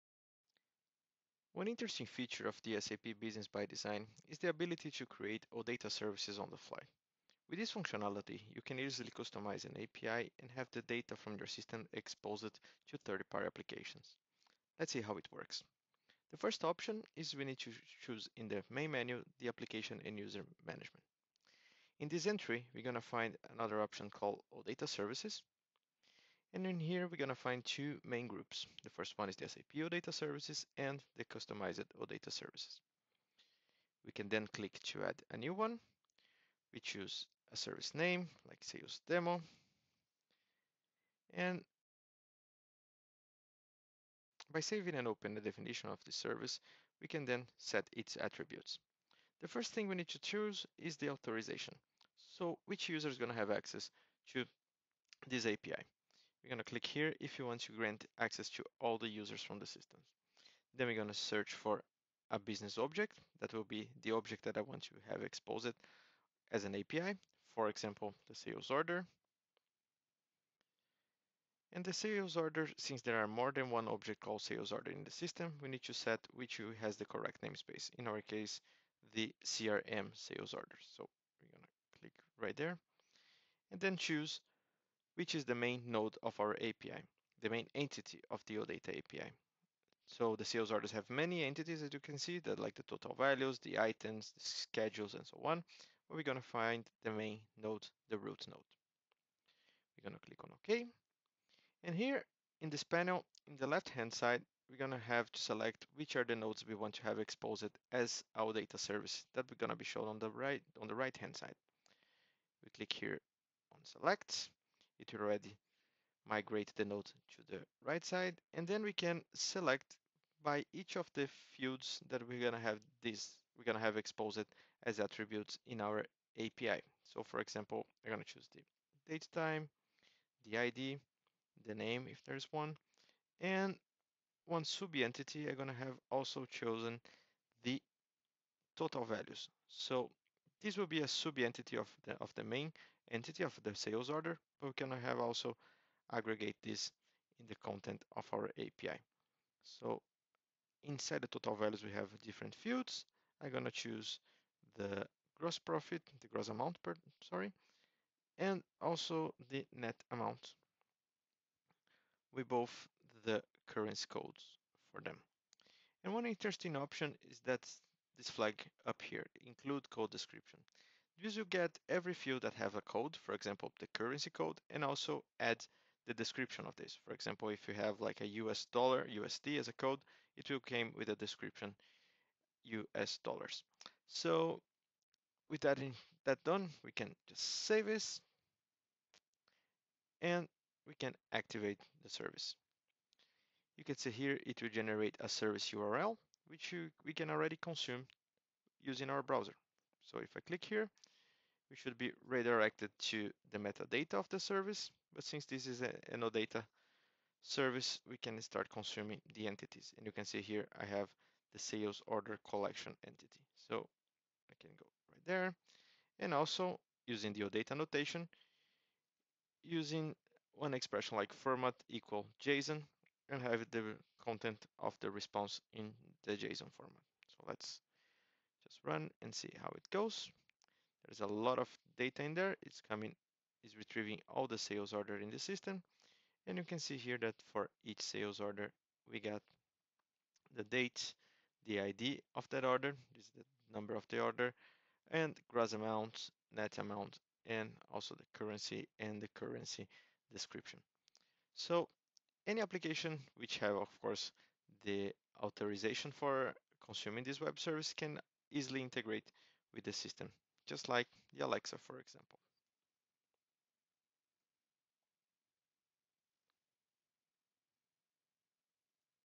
SAP Business by Design - How to Create an OData Service
A simple demo of how to expose SAP Business By Design data as odata services